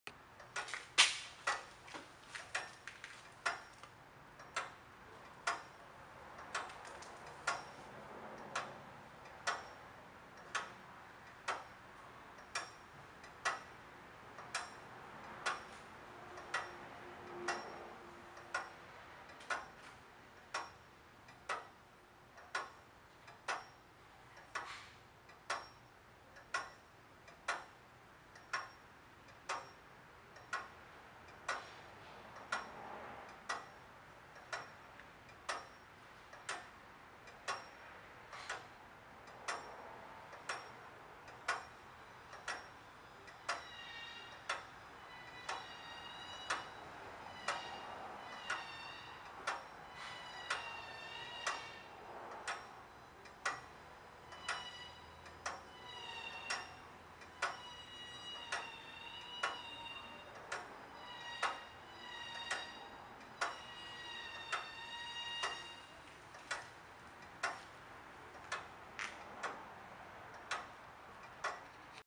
Restored Howard Street Clock Movement
Restored Howard Street Clock Movement in test in our shop. Clock movement also has an electric wind.